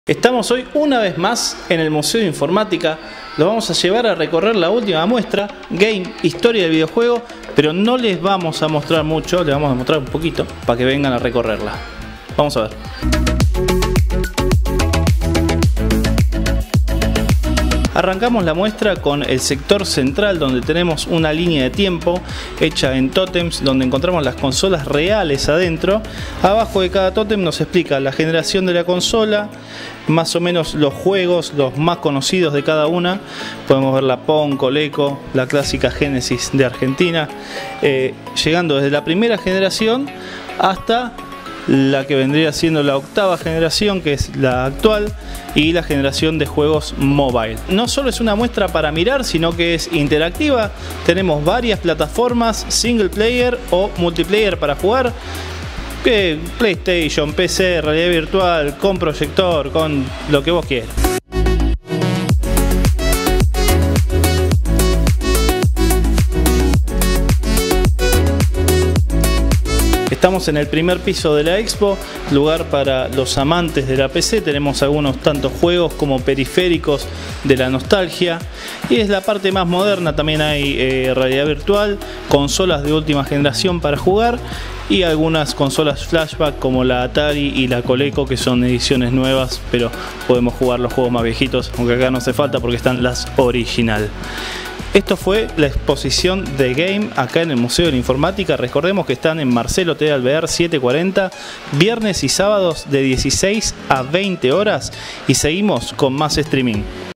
Expo GAME en el Museo de Informática
¡Estuvimos en la expo Game que realiza el Museo de Informática y te mostramos parte de ella!

La exposición se realiza en Marcelo T. De Alvear 740, CABA.